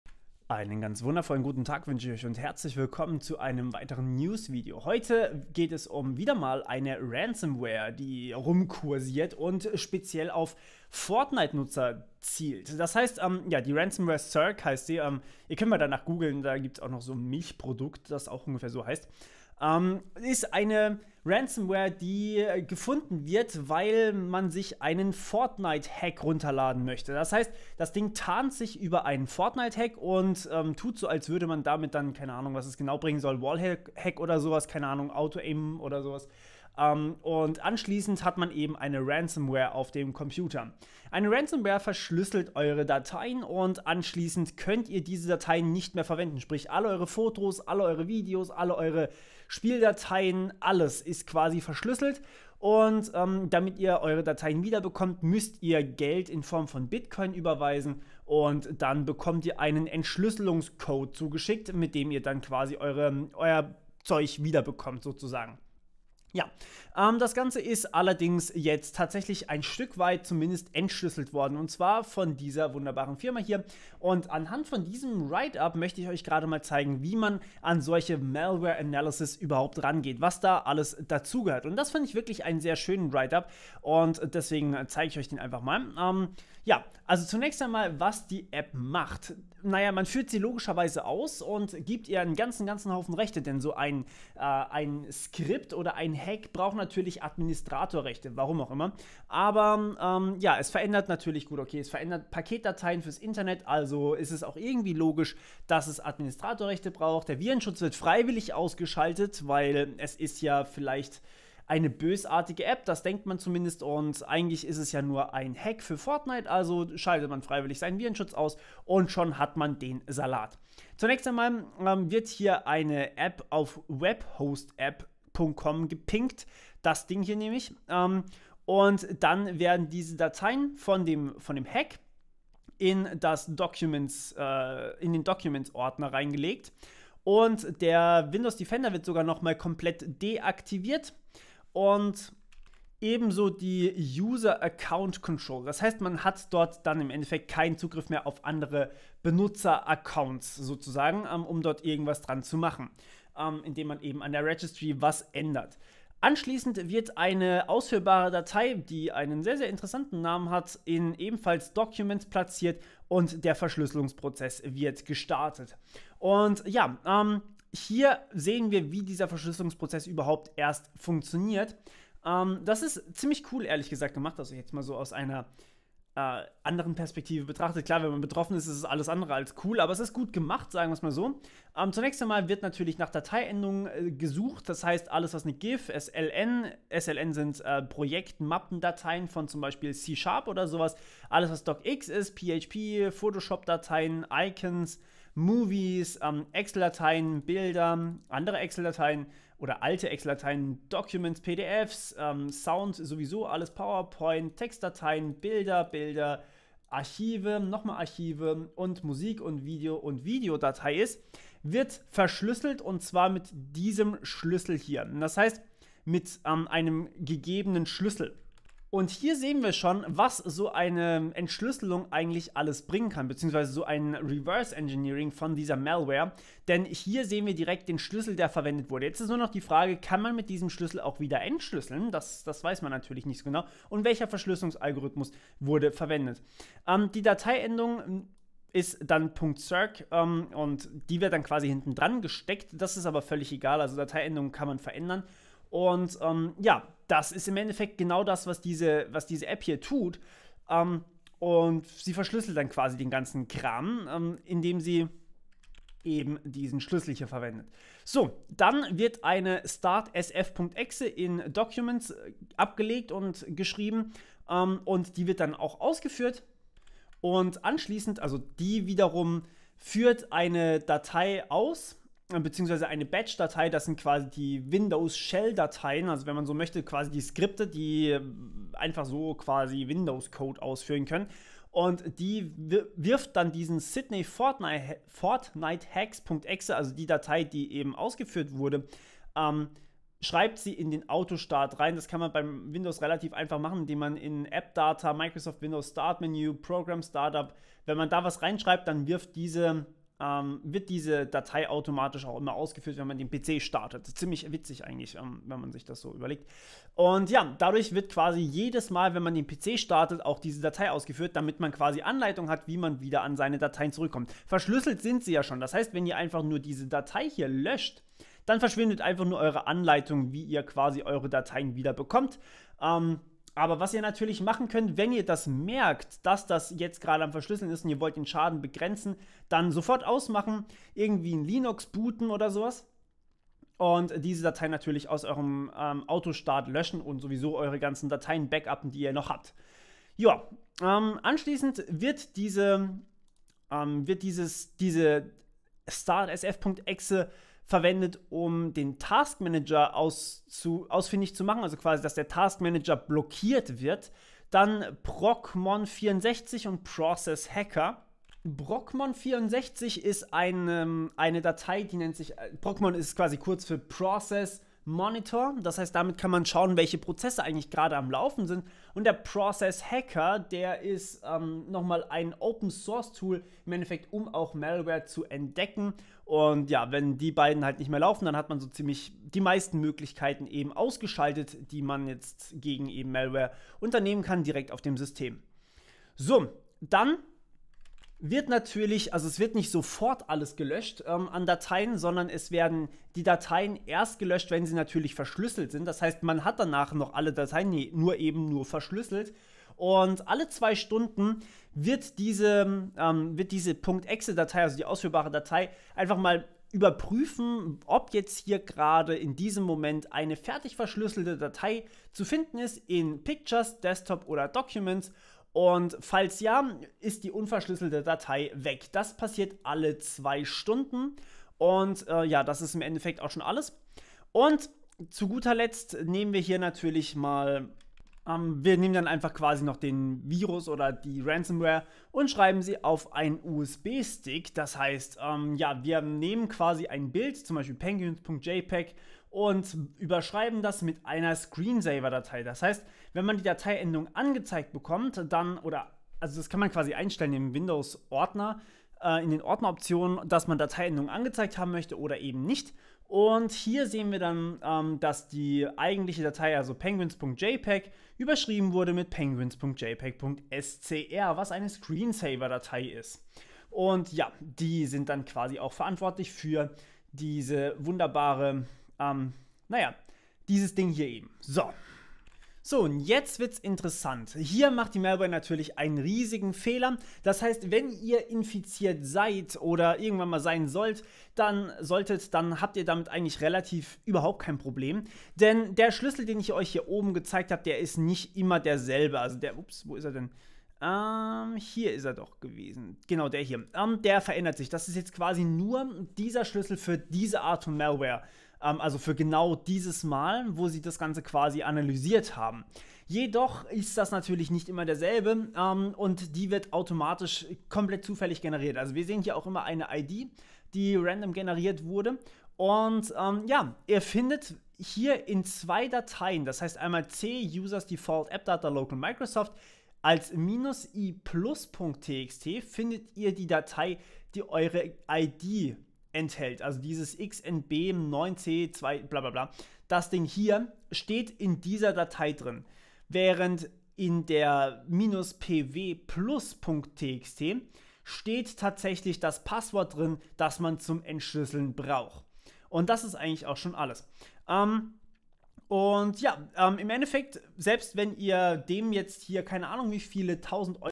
Syrk Ransomware zielt auf Fortnite Spieler - wie ihr den Virus loswerdet
Angewandte Informatik * *Meine Website mit allen anderen Kanälen und Newsletter* *